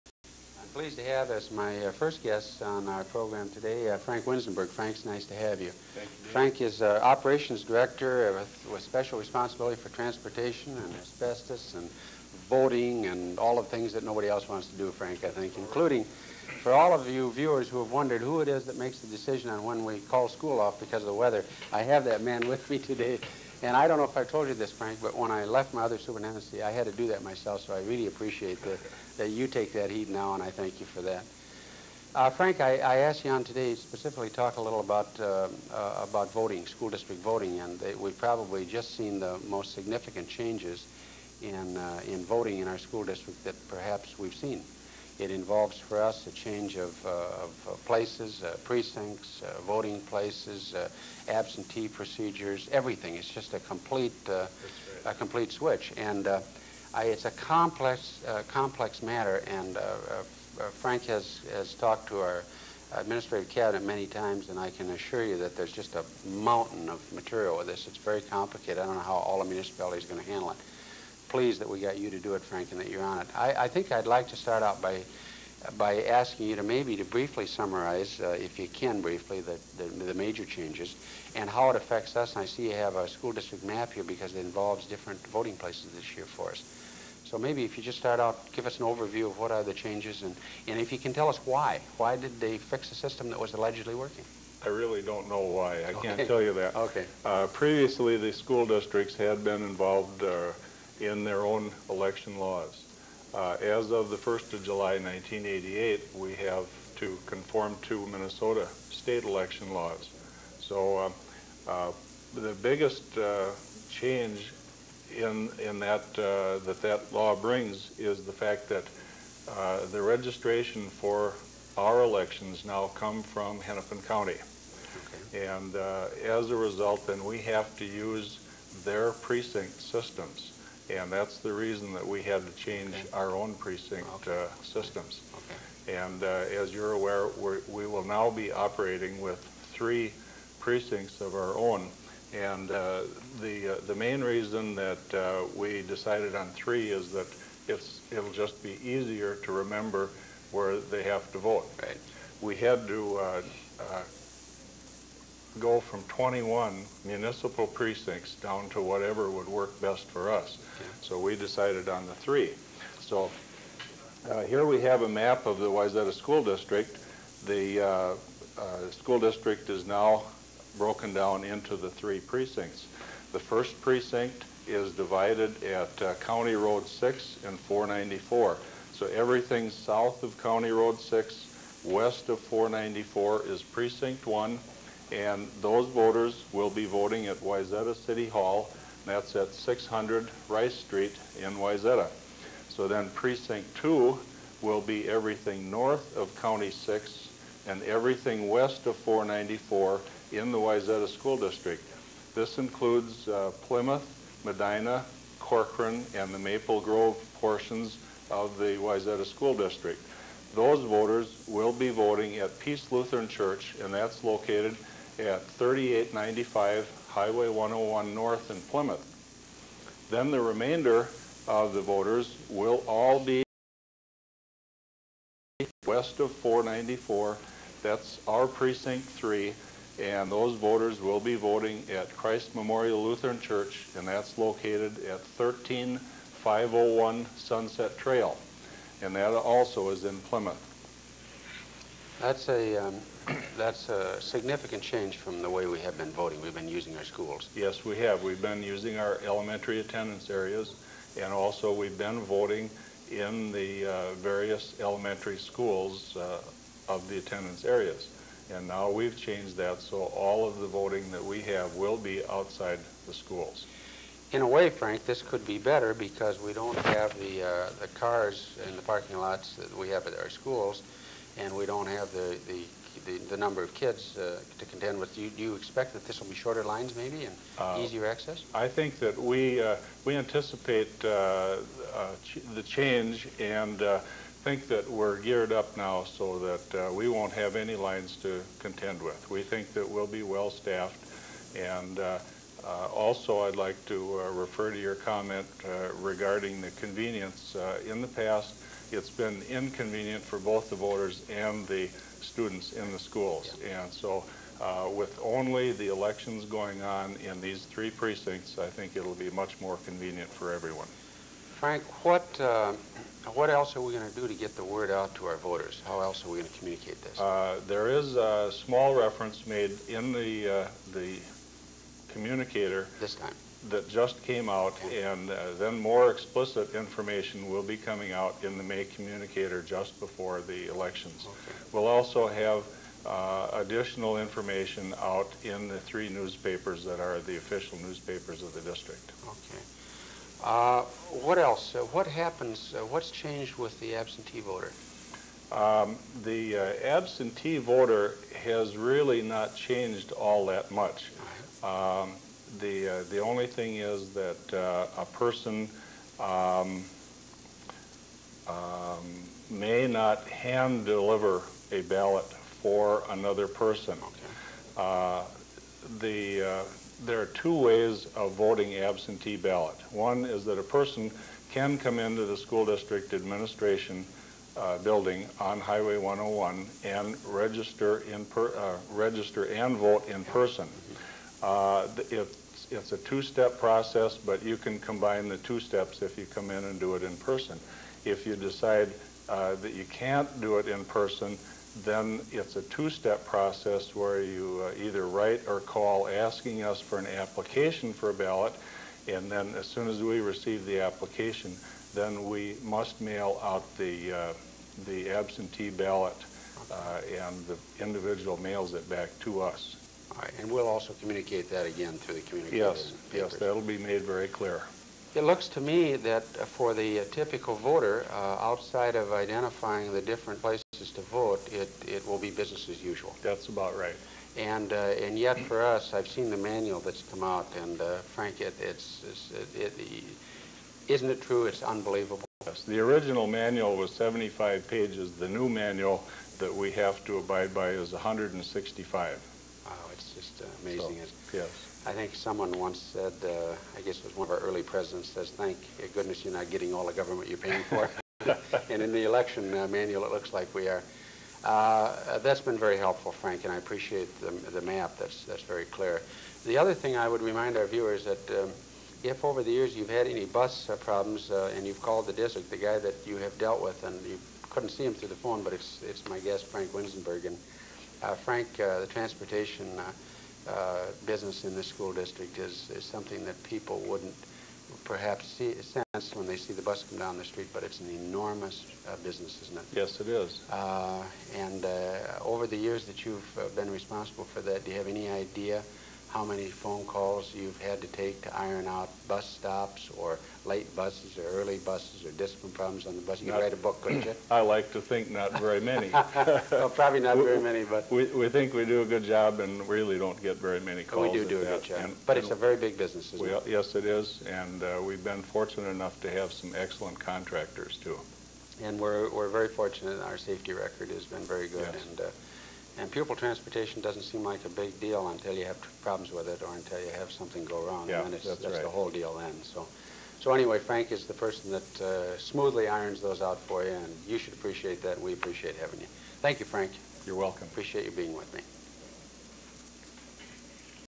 Frank E Winzenburg Interview - Wayzata School Superintendent David Landswerk "Around District 284"
In this interview from the late 1980s, Wayzata Minnesota Superintendent David Landswerk talks with Frank E. Winzenburg about his job coordinating school transportation and calling off classes on snow days.